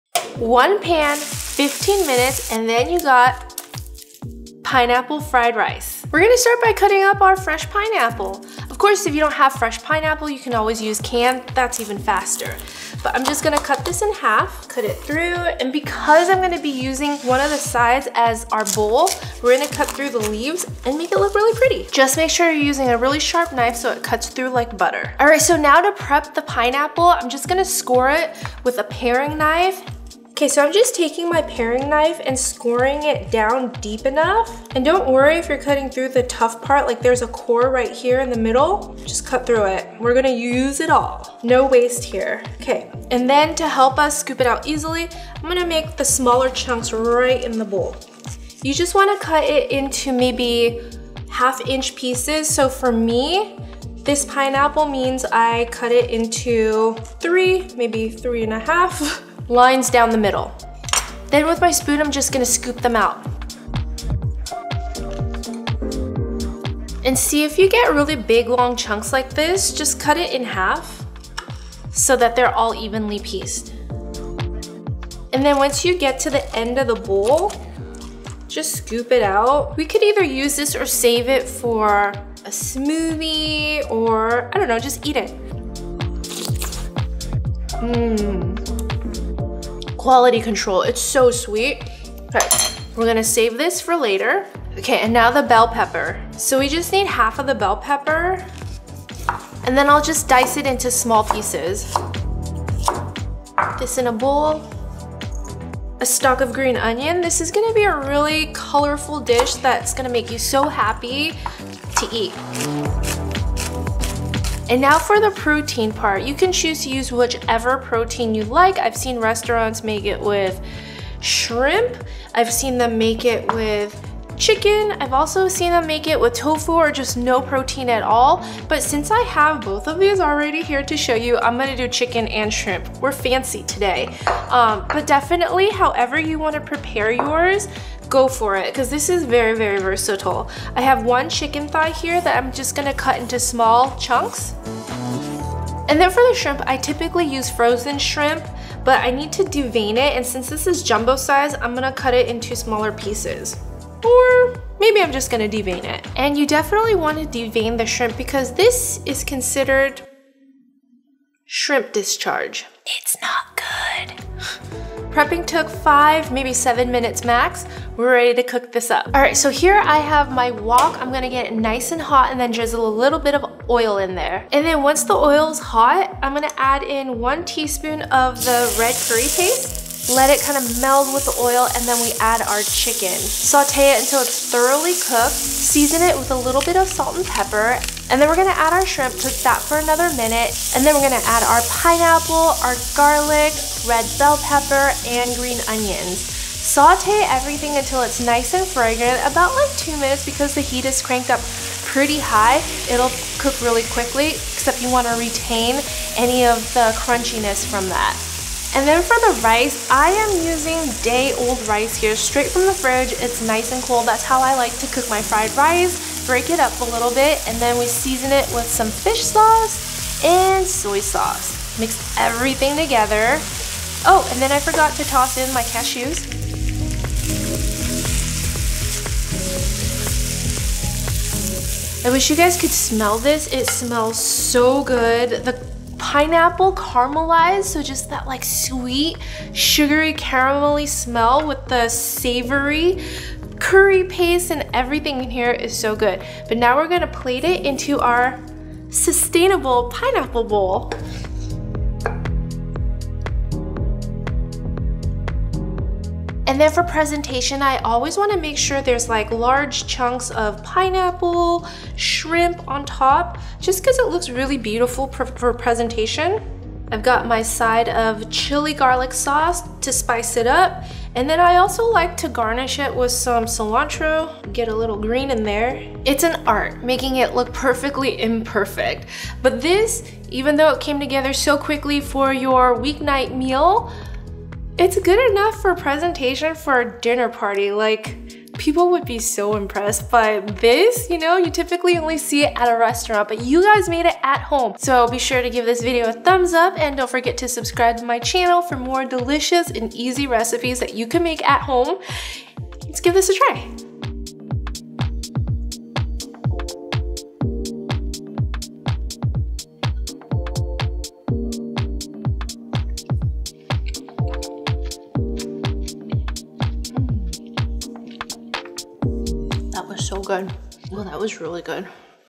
PINEAPPLE FRIED RICE in 15 Minutes!
Today we're making Pineapple Fried Rice in just one wok. And it takes just 15 minutes, it comes together super fast! Recipe Below
➜ Mailing Address:
26500 Agoura Rd
Ste 102

PINEAPPLE FRIED RICE RECIPE:
2 cups fresh pineapples, cut into 1/2 inch chunks
1/2 a bell pepper, diced
1 stalk green onion
2 cloves garlic, minced
1 teaspoon red curry paste
1 cup protein of choice
3 cups day-old rice
1 tablespoon fish sauce
2 teaspoons soy sauce
1/4 cup roasted cashews

In a large wok, heat up with a drizzle of oil. Let it get hot and add in the curry paste. If cooking with chicken, add the chicken cut into small pieces, and stir fry for 4-5 minutes until cooked through. If using shrimp, cook for a minute until pink and remove / set aside. If cooking shrimp and chicken, then leave in and add the pineapples, bell pepper, garlic, and green onion.

Nestle the shrimp on top of the veggies to slow down the cooking process.
Once the pineapple starts to caramelize for (about 3 minutes), add the cold rice and break it up a bit. Add the fish sauce and soy sauce, the cashews, mix well and serve.

00:00 Intro
00:06 Prepping the Fruit & Veg
01:54 Prepping the Protein
02:48 Cooking it Up
04:06 Special Plating